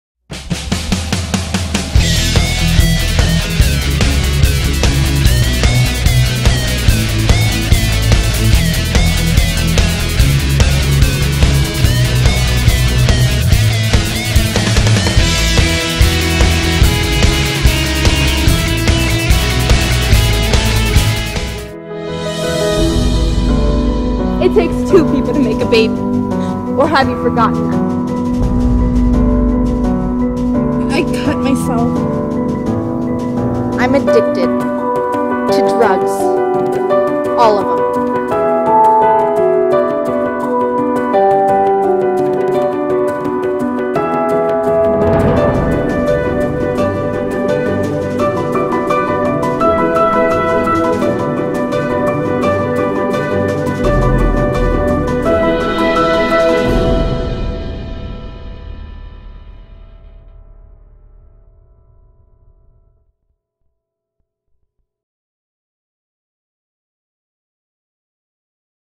"Dead Serious...About Life" Coming to Chillicothe High School
Trailer for "Dead Serious...About Life" that will be performed at Chillicothe High School in Chillicothe, OH on Saturday 3/2 @ 6:00 pm & Sunday 3/3 @ 3:00 pm.  A MUST SEE for teens.